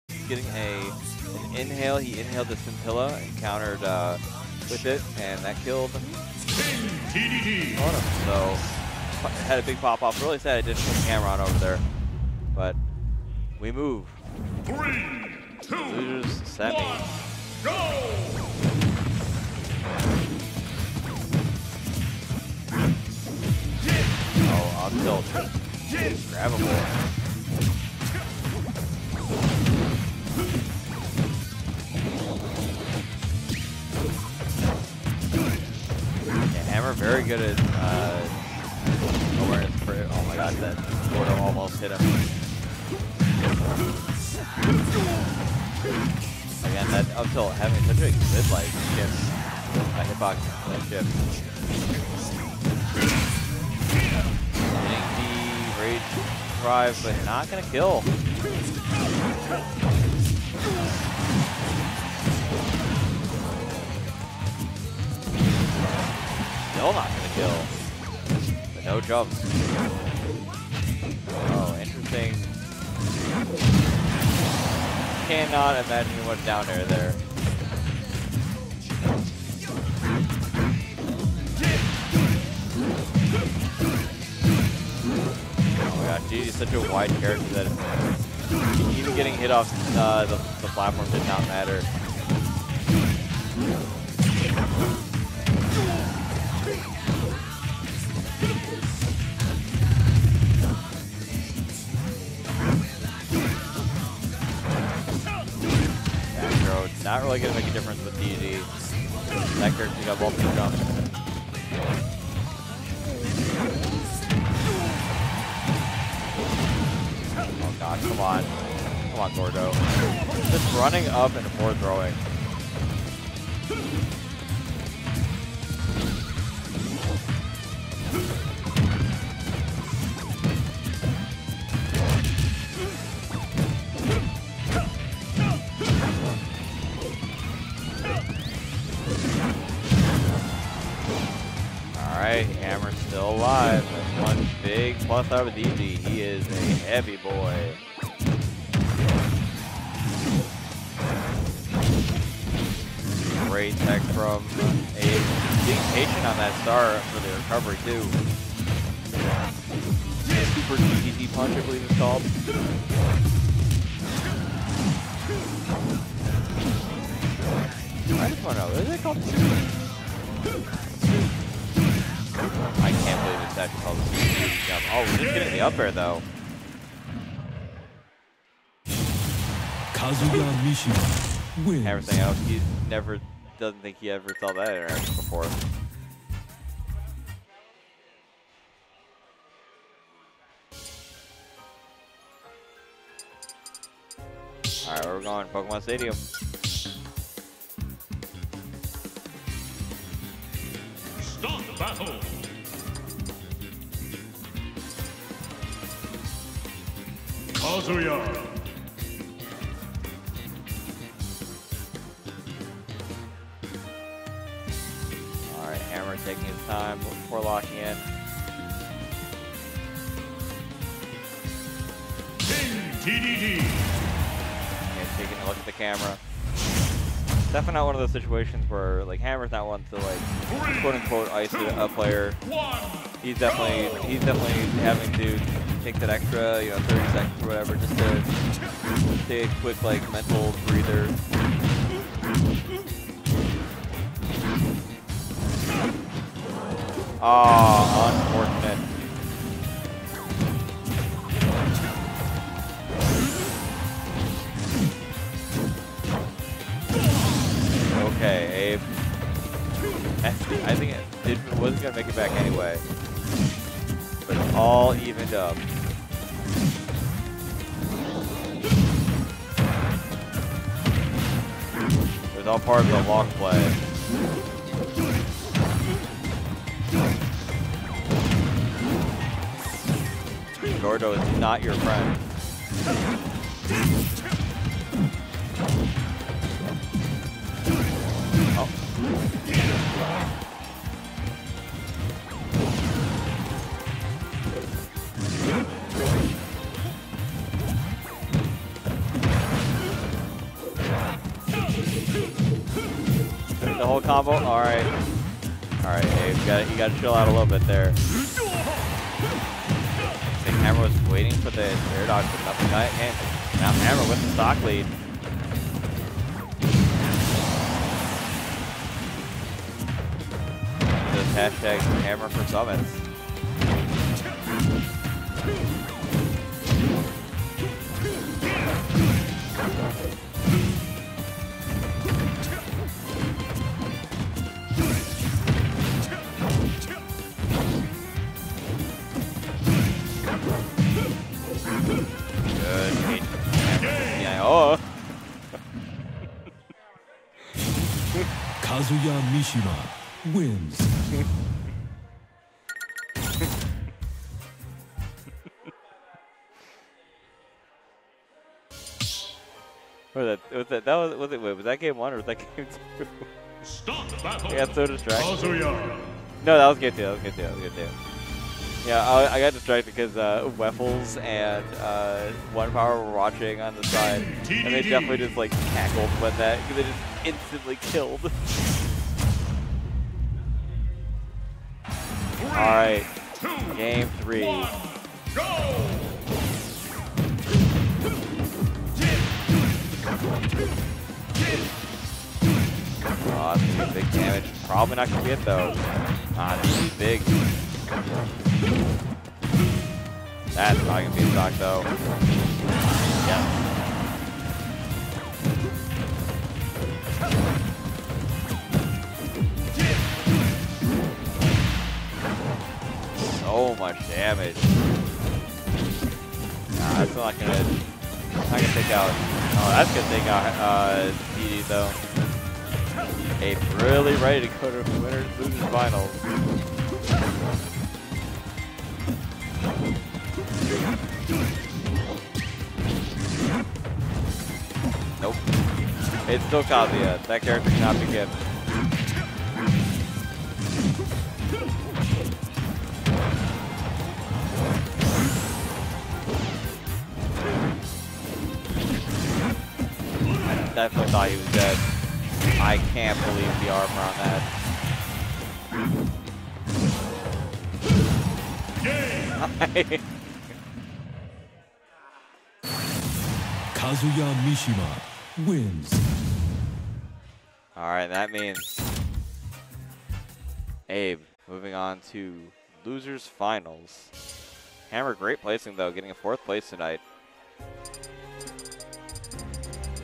abezuraa (Kazuya) vs Hammer (Dedede) - Ignition 357 LOSERS SEMIS
Ignition 357 - September 4, 2024

Venue Location: 8125 Skokie Blvd, Skokie, IL 60077

Follow @UR_Tournaments, @Xyless, and @smash_pacman for more live updates!